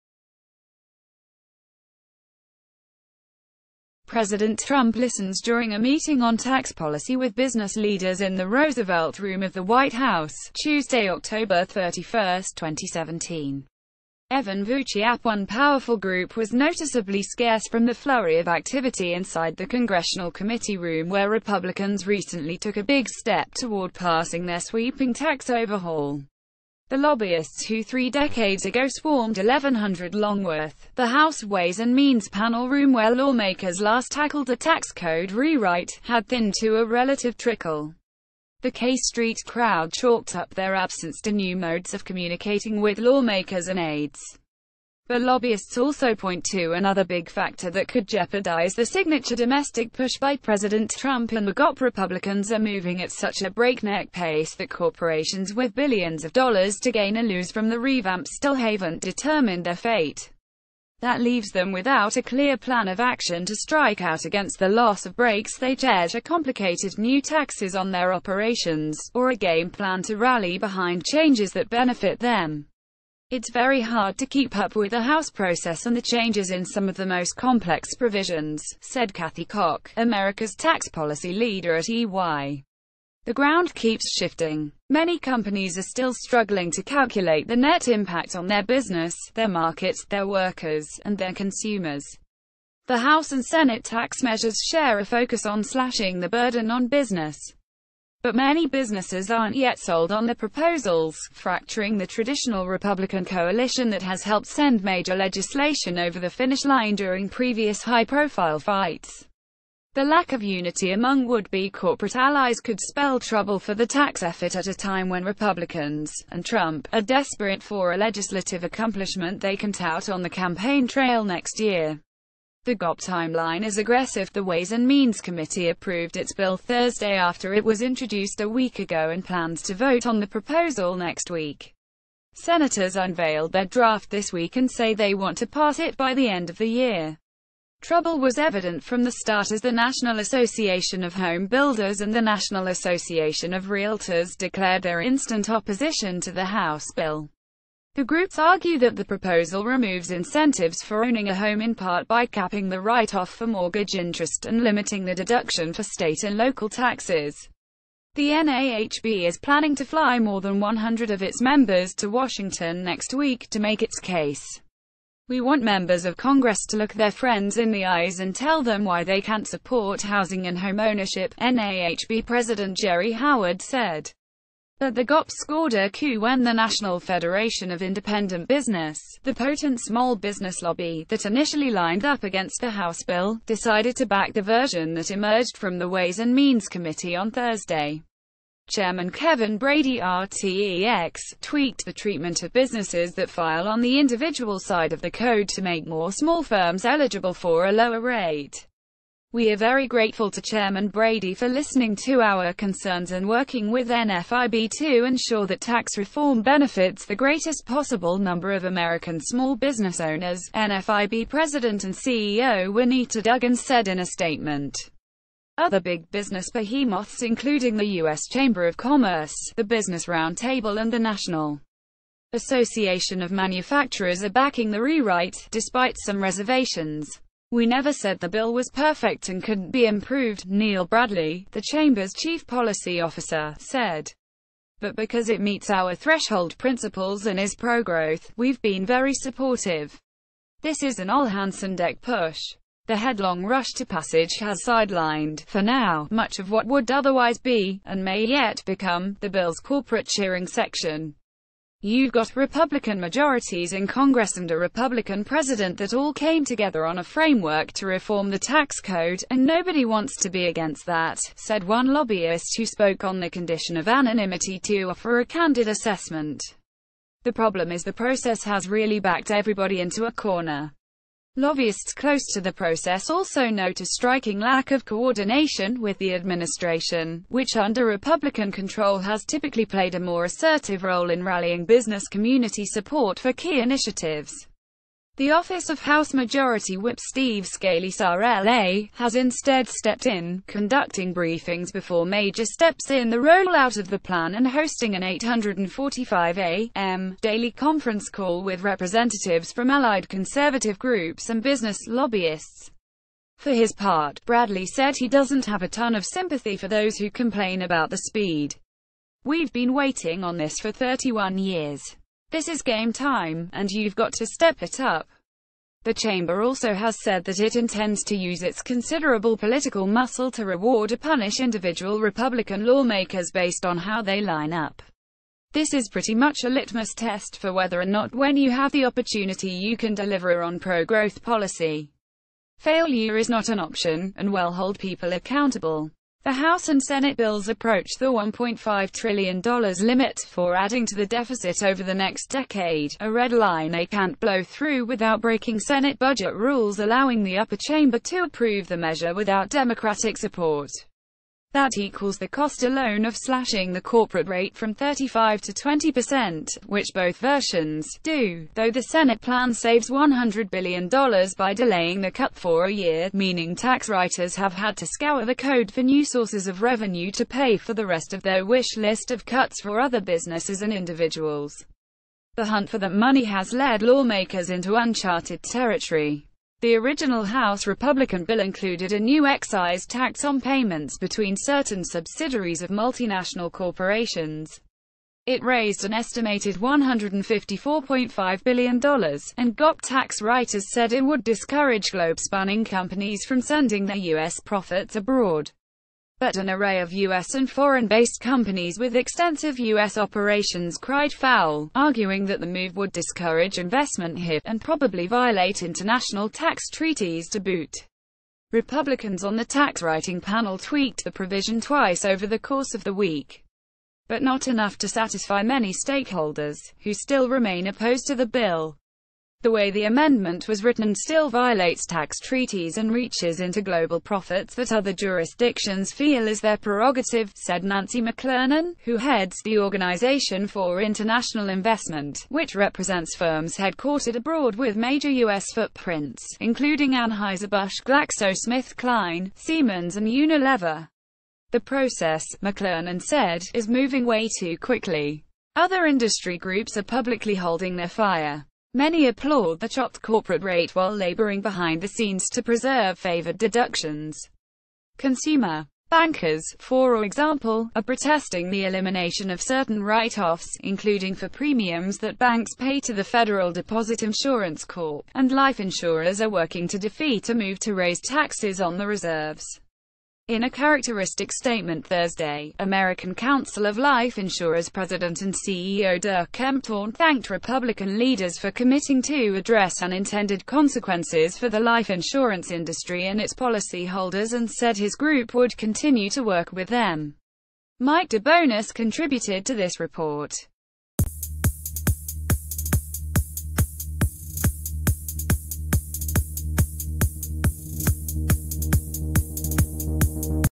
Business groups typically allied with republicans not all on board fast tax train | UK News Express
President Trump listens during a meeting on tax policy with business leaders in the Roosevelt Room of the White House, Tuesday, Oct. 31, 2017. Evan VucciAP One powerful group was noticeably scarce fro...